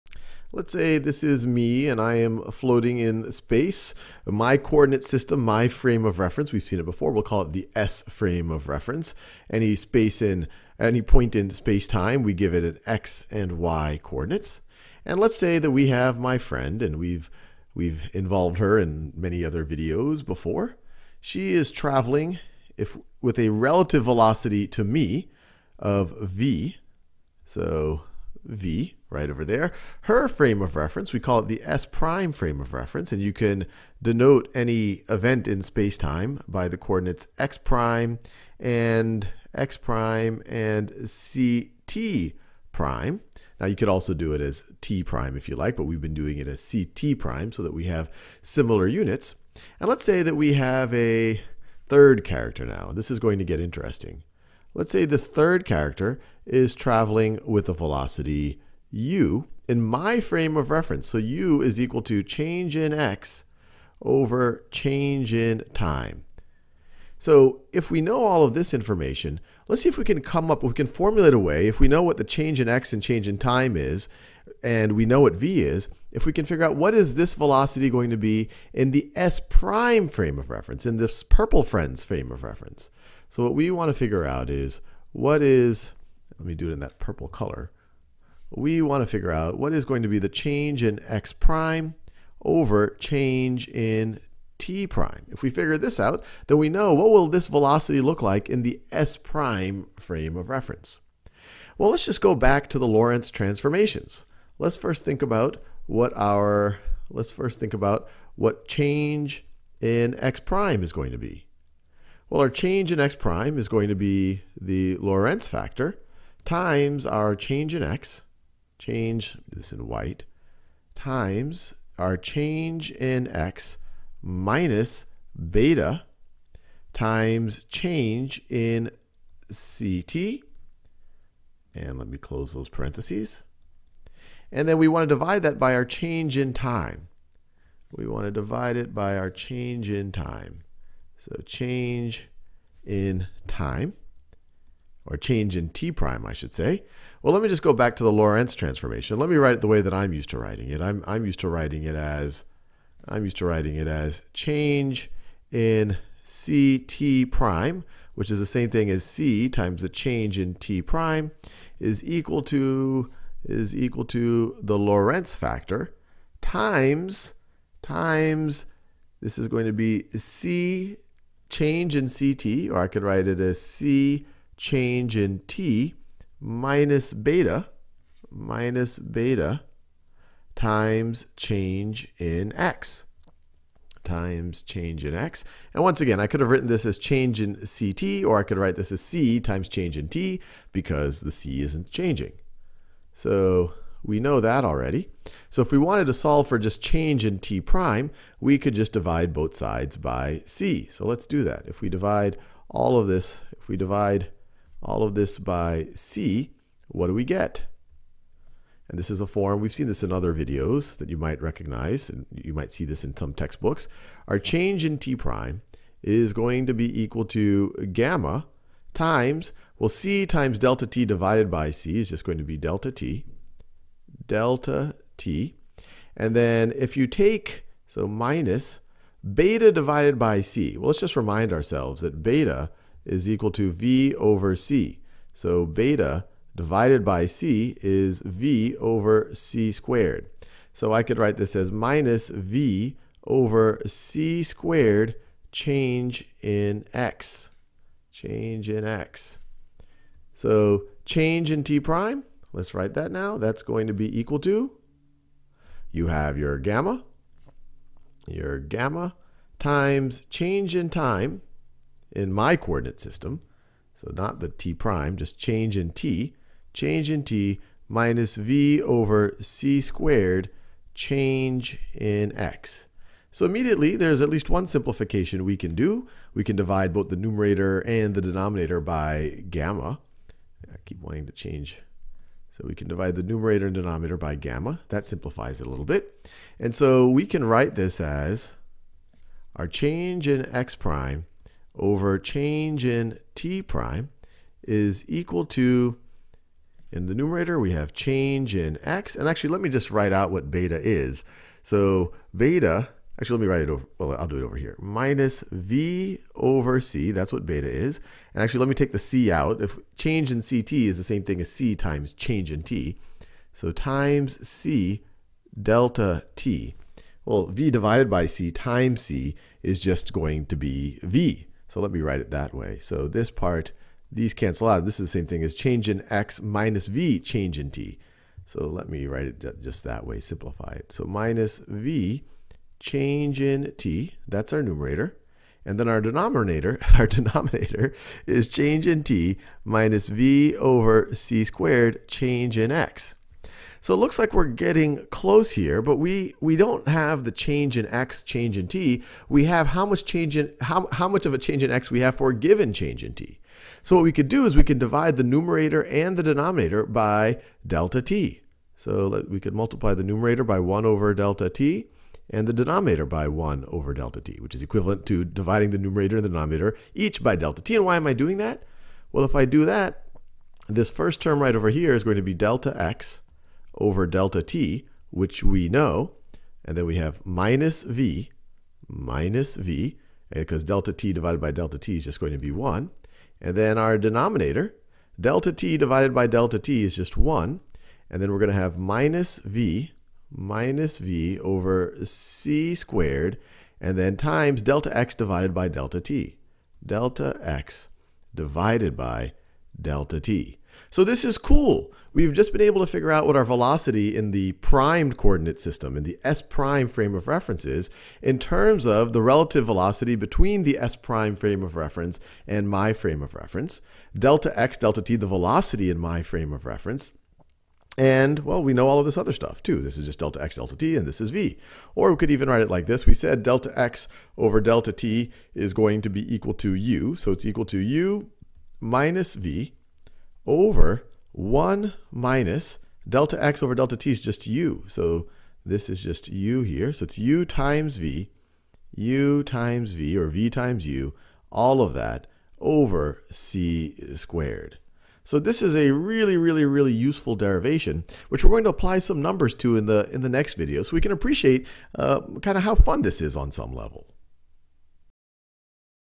Einstein velocity addition formula derivation | Special relativity | Physics | Khan Academy
Courses on Khan Academy are always 100% free. Start practicing—and saving your progress—now

Einstein velocity addition formula derivation.